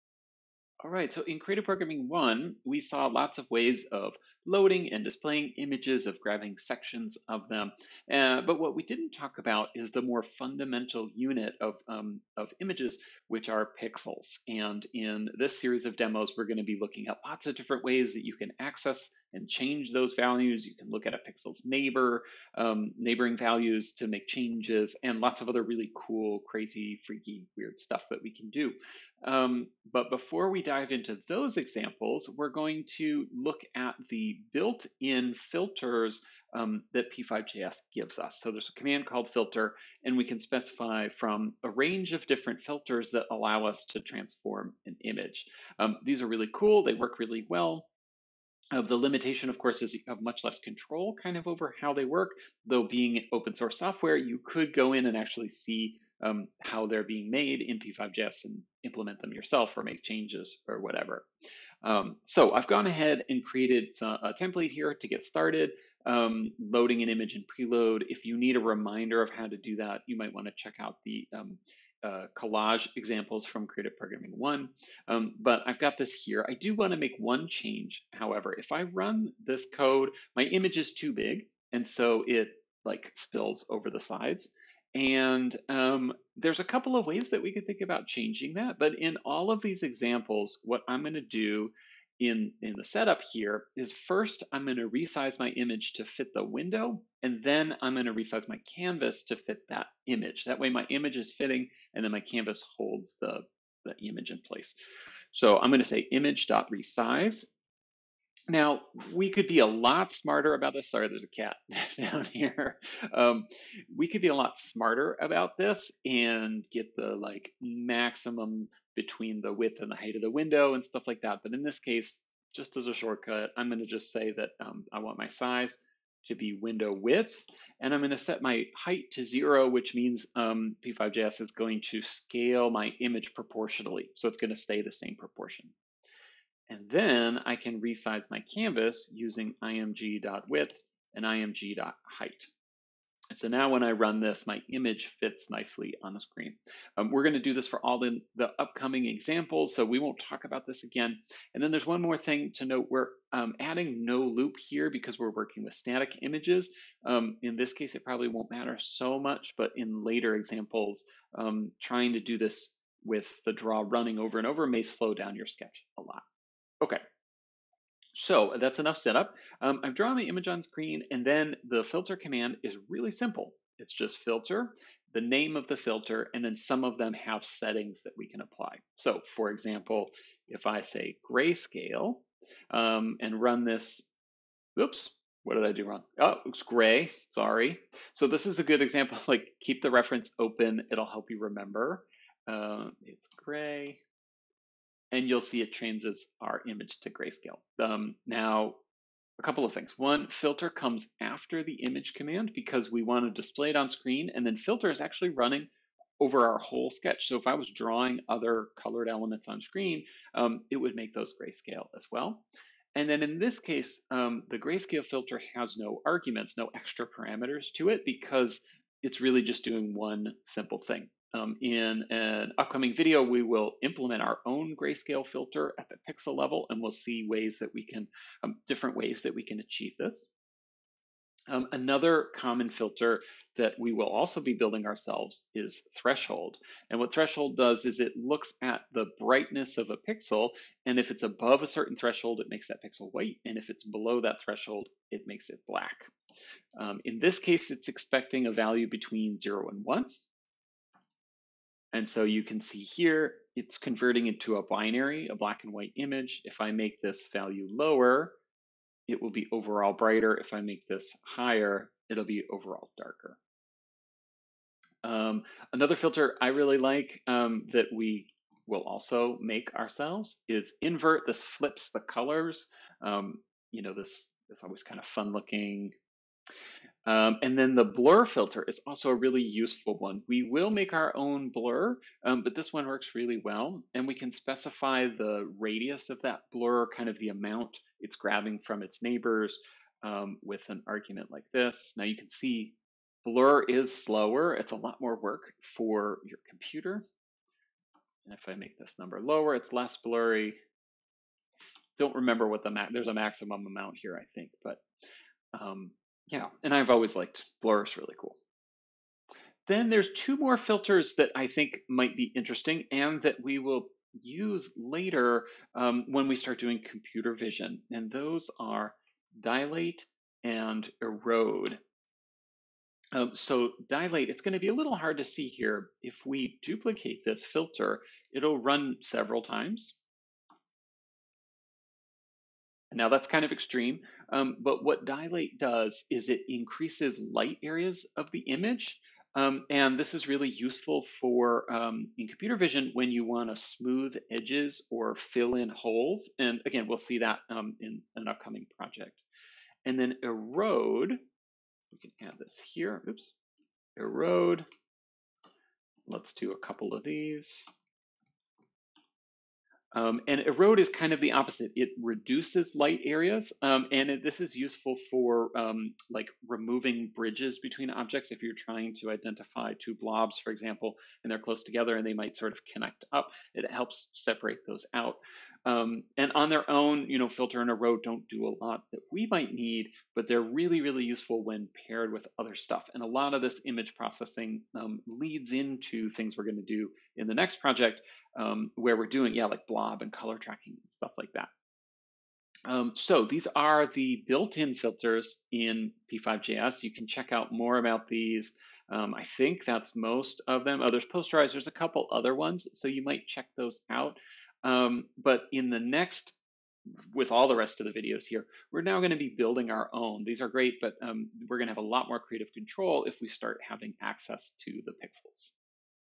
CP2: Built-in Filters with p5.js – Image Filters
p5.js comes with some pre-built filters that change the pixels onscreen, ranging from converting a color image to grayscale to smooth blurring. The filters can be used for artistic effect on their own (though, like the filters in Photoshop, they tend to look a bit cheesy and pre-made) but are better used to prepare images for further processing.

Later, we'll implement a few of these ourselves. We'll also see some of these filters used in computer vision to prepare images for object detection. This is also a crucial step in machine learning to make training and feature extraction easier.

ORDER MATTERS!
Like signal processing in audio, the order in which we apply filters matters. If we first blur, then threshold, we'll get a very different result than in the opposite order. Thinking about how you apply the filters (or at least trying things and see what happens) will be important to get the best results.

CHALLENGES
❓ Can you make the filter parameters interactive using the mouse or keyboard?
❓ Can you chain several filters together to make something more interesting than a single on by themselves? For example, try blurring before running a threshold filter.